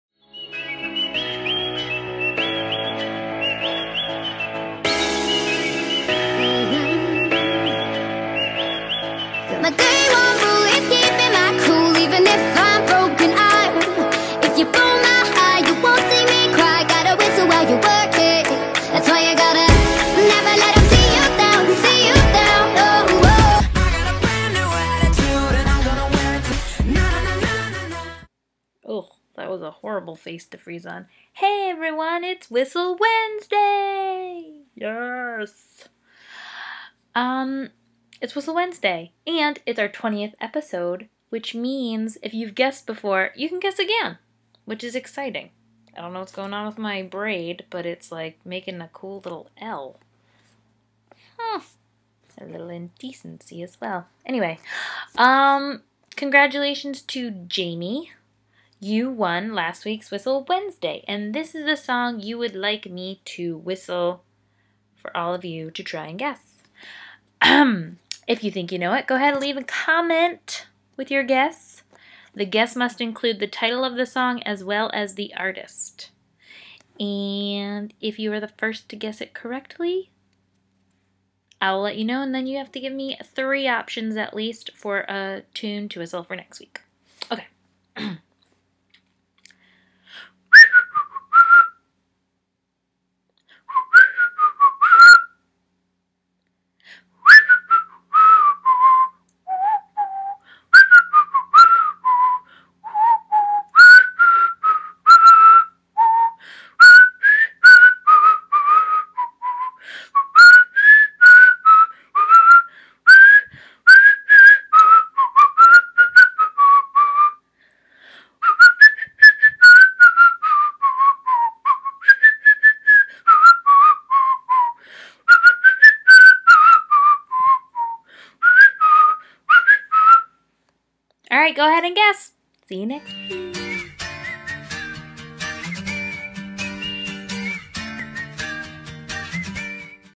Whistle Wednesday Episode 20
Seana whistles a song and viewers have to try to guess the correct tune first!

**Episodes were previously recorded in 2015